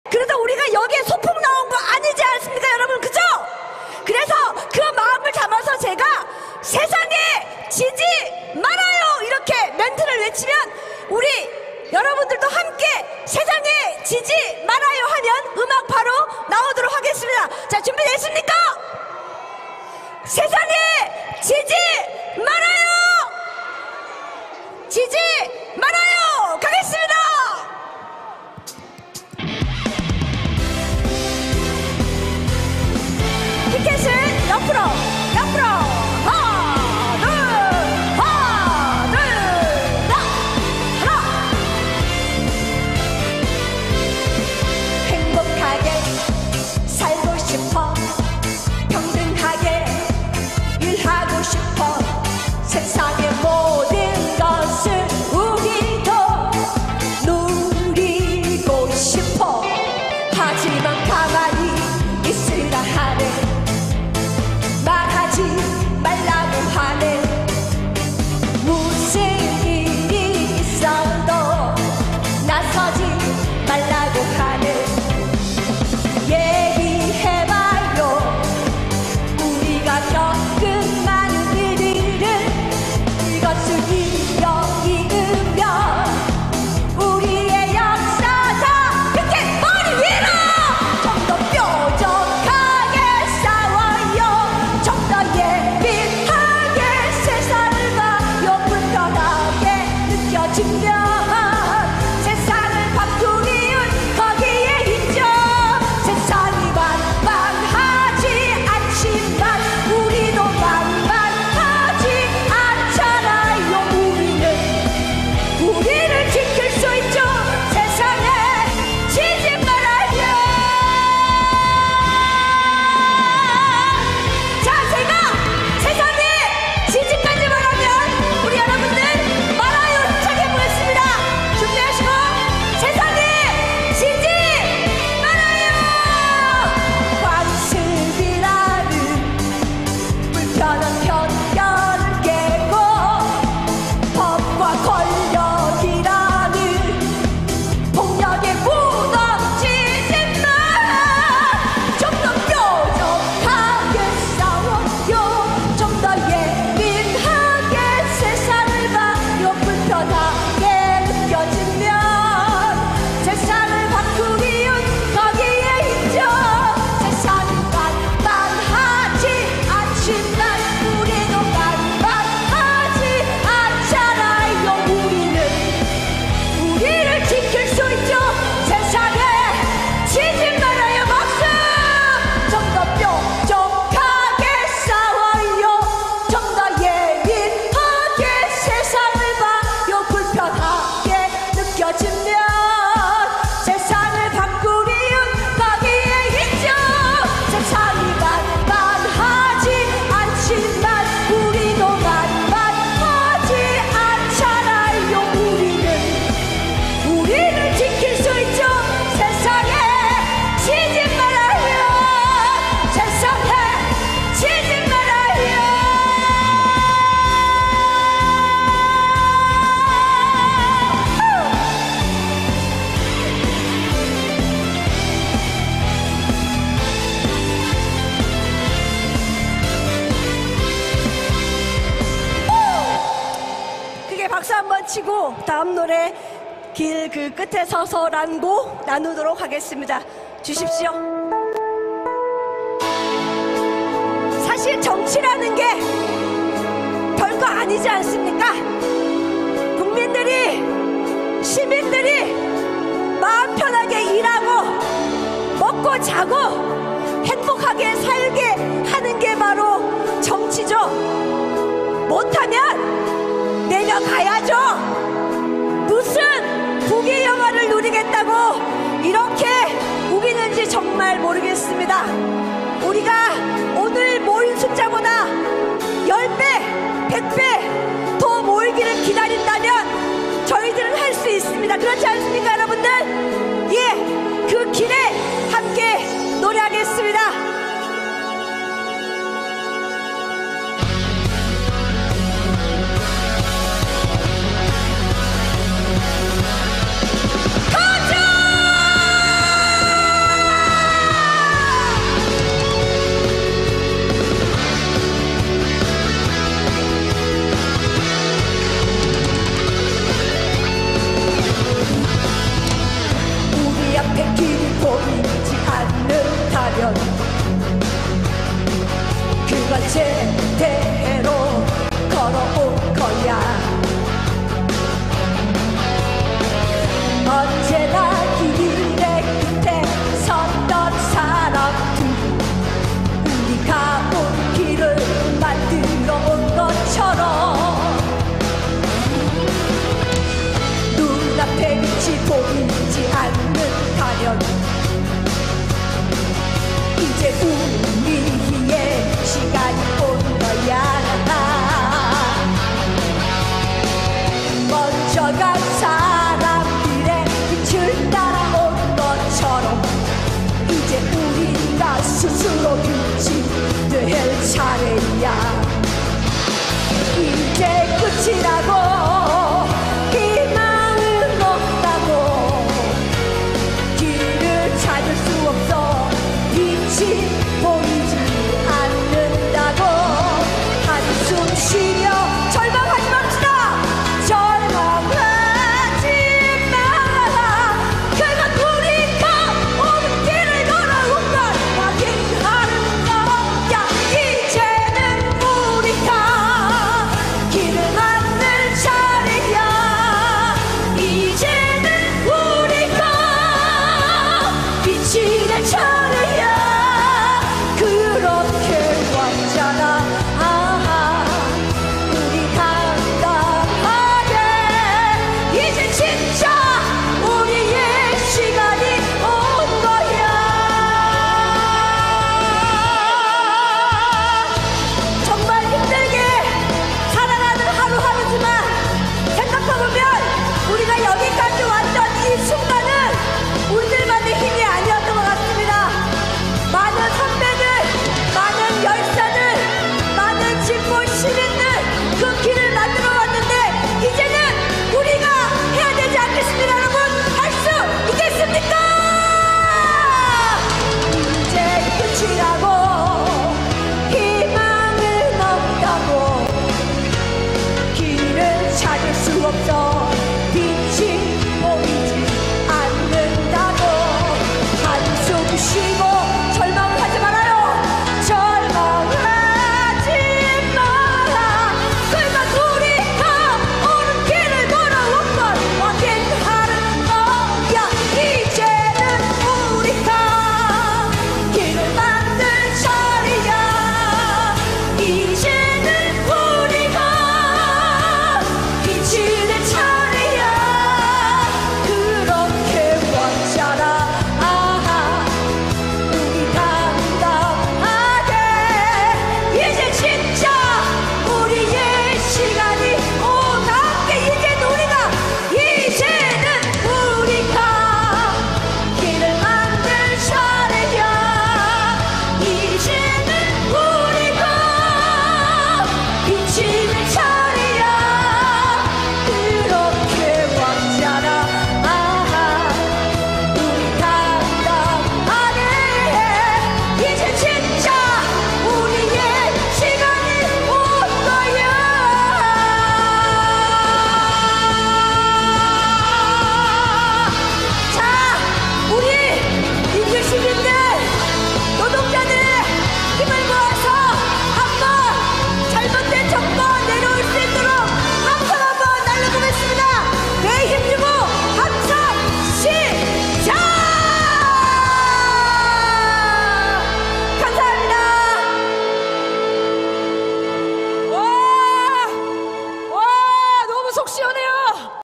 가수 지민주 "이제 진짜 우리의 시간이 온거야~!" 속시원한 공연 미쳤다!
4년 전 시작된 촛불항쟁은 현재 진행형입니다.